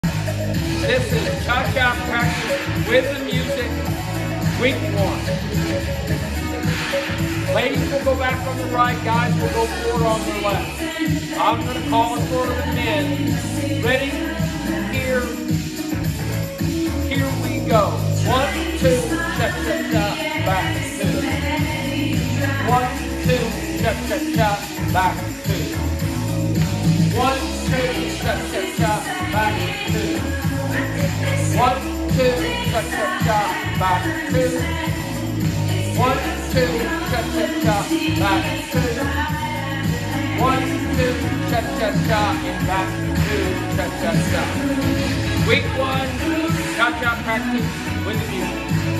Cha Cha Practice with Music for Virtual Dance Lesson 1 with Frank Hancock
Practice Cha Cha with music as Frank calls out the steps.